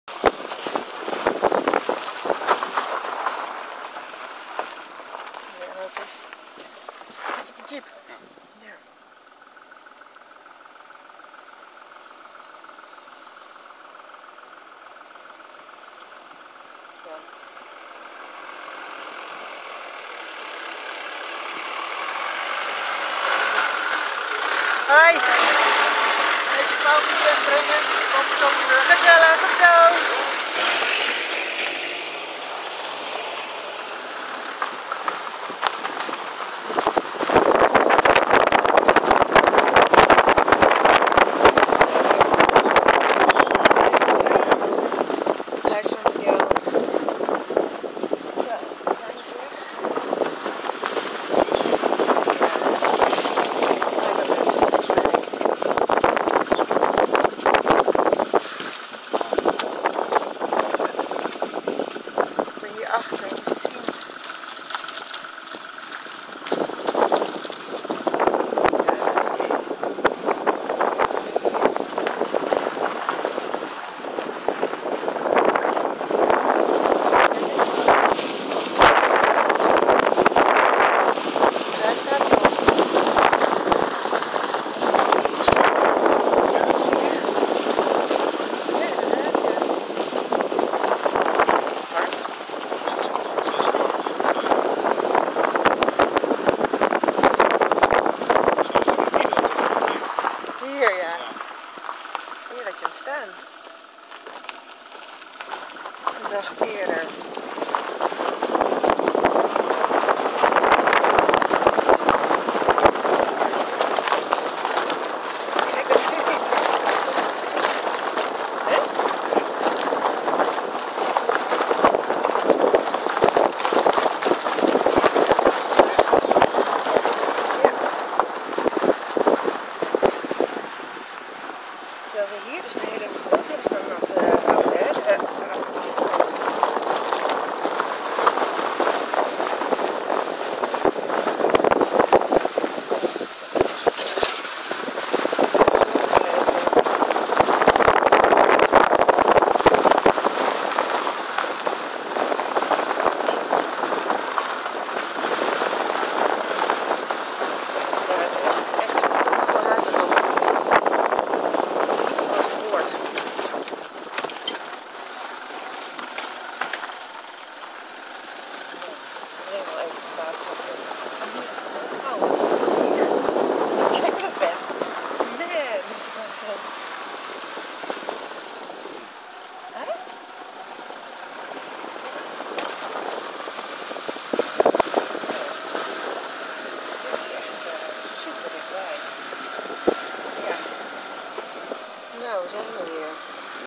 Day after the fire near Moinhos do Dão 4/4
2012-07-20

One day after the first fire (and the first series of video's) a second fire hit the area around Villa Garcia and the beautiful Moinhos do Dão. This video was shot a day later, when it was safe to return to the Moinhos.
Both luck and the hard work of the volunteers at the Moinhos and the Corpo de Bombeiros, have saved this beautiful oases of tranquility, which is now, surrounded by a scorched earth an even bigger oases of wonder.